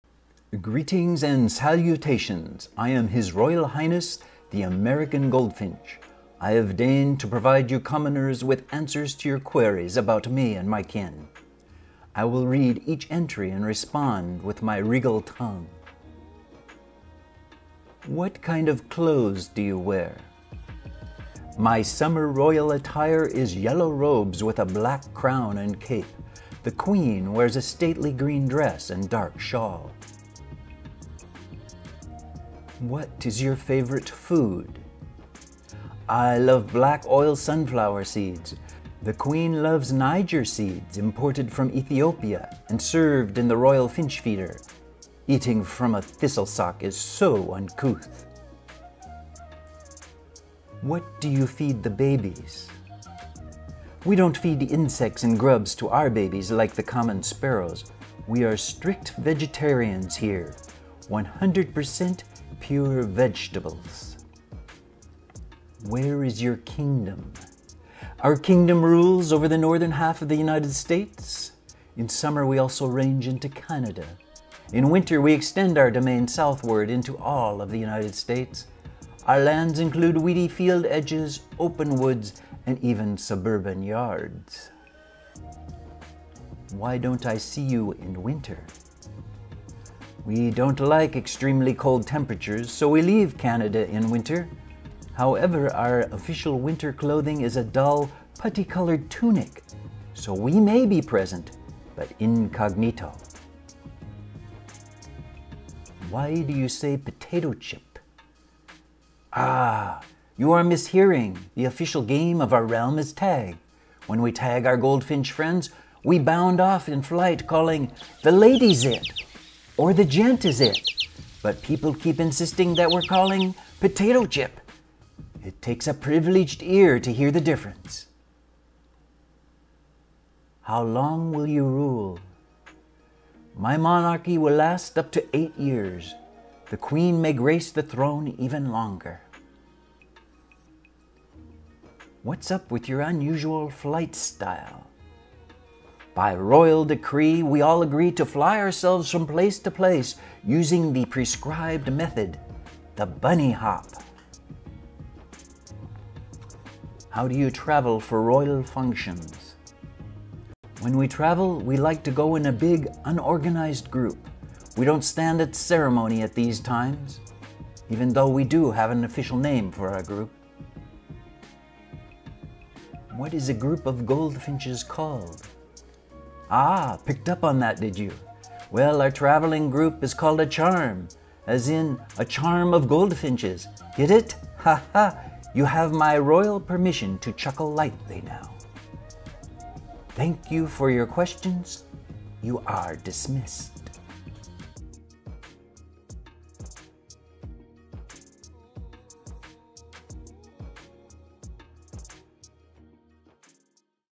Interview With American Goldfinch: 10 Facts You Want to Know
In this video His Royal Highness, The American Goldfinch, shares his answers to your frequently asked questions about American Goldfinches. Learn 10 Facts about American Goldfinches.

Fact #1: Breeding male American Goldfinches are bright yellow with black crown, wings and tail.
Fact #2: At your feeder American Goldfinches eat black oil sunflower seeds and Niger seed.
Fact #3: American Goldfinches feed regurgitated seeds to their nestlings.
Fact #4: American Goldfinches range across the United States and into Canada in summer.
Fact #5: American Goldfinches molt into a dull-colored winter plumage.
Fact #6: The flight call of American Goldfinches has been described as a lilting “Potato Chip!”
Fact #7: American Goldfinches can live 8-10 years.
Fact #8: American Goldfinches fly with deeply undulating bursts of flapping.
Fact #9: American Goldfinches are often found in flocks of 10-200 birds.
Fact #10: A flock of goldfinches is called a “Charm of Goldfinches.”

Chapters
0:00 Intro
0:16 What does an American Goldfinch look like?
0:31 What do American Goldfinches eat?
0:49 What do American Goldfinches feed their young?
1:05 Where do American Goldfinches live?
1:25 Where do American Goldfinches go in the winter?
1:44 What do American Goldfinches sound like?
2:09 How long do American Goldfinches live?
2:21 What is the flight pattern of a goldfinch?
2:35 Do American Goldfinches travel in groups?
2:51 What's a group of Goldfinches called?

Enjoying Nature
Improve Your Birding Skills
Bird Identification
Facts About Single Bird Species
Short Nature Clips
Feeder Birds of Each State
Q&A: Frequently Asked Questions

Attribution. Thank you to these recordists for photos, videos, and audio recordings.

Flight Call
Jonathon Jongsma
Xeno-Canto XC 428425